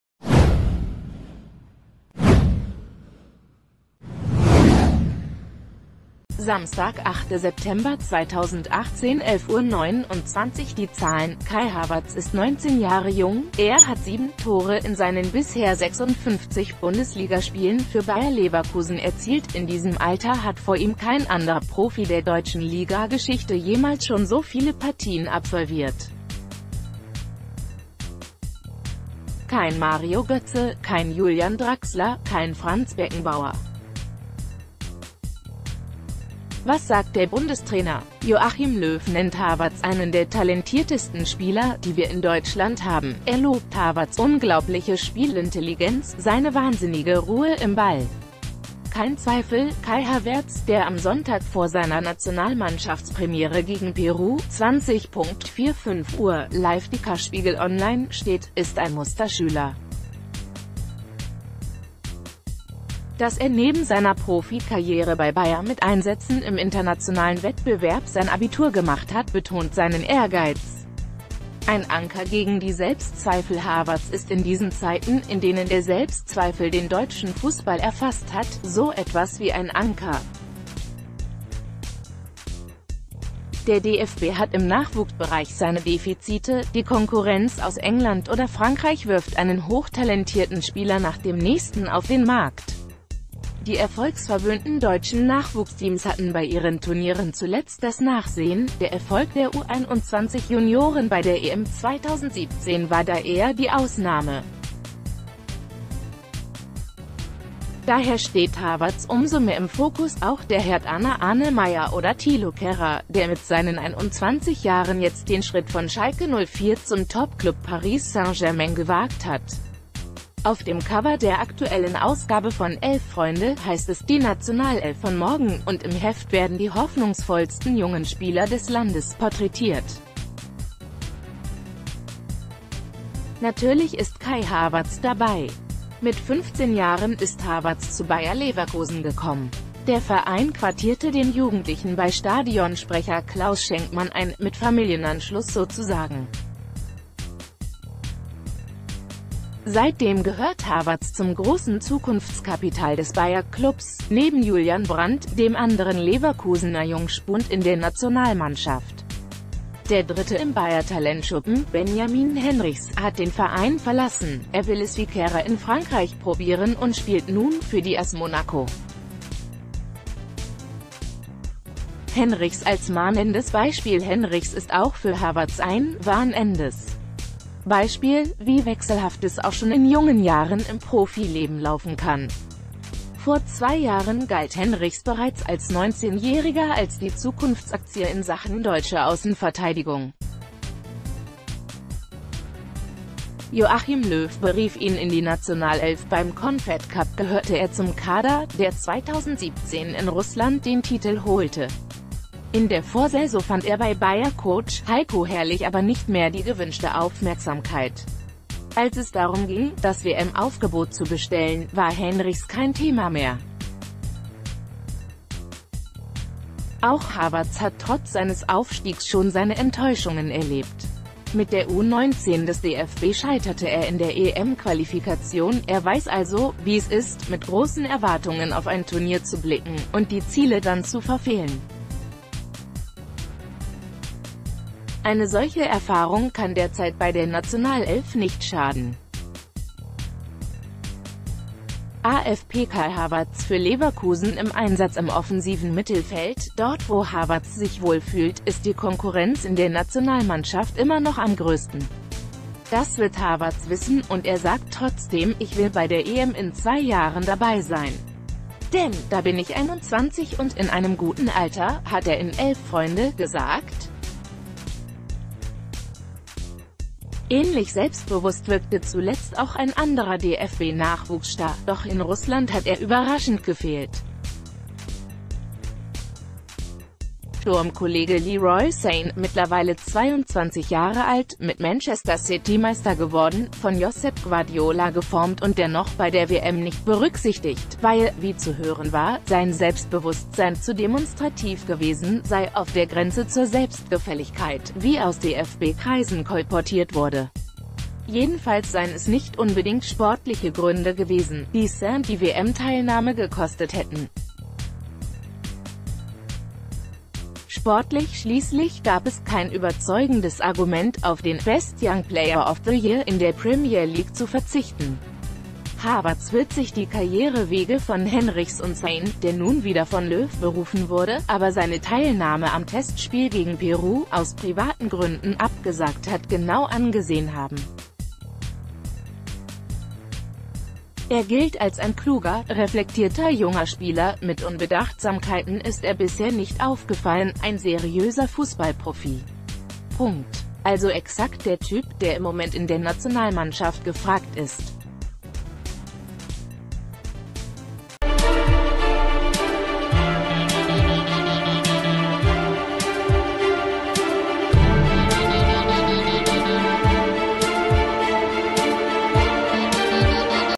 Kai Havertz vor DFB-Debüt: Er soll Deutschlands Zukunft sein
Samstag, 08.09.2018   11:29 Uhr Die Zahlen: Kai Havertz ist 19 Jahre jung, er hat sieben Tore in seinen bisher 56 Bundesligaspielen für Bayer Leverkusen erzielt, in diesem Alter hat vor ihm kein anderer Profi der deutschen Ligageschichte jemals schon so viele Partien absolviert. Kein Mario Götze, kein Julian Draxler, kein Franz Beckenbauer. Was sagt der Bundestrainer? Joachim Löw nennt Havertz "einen der talentiertesten Spieler, die wir in Deutschland haben", er lobt Havertz' "unglaubliche Spiel...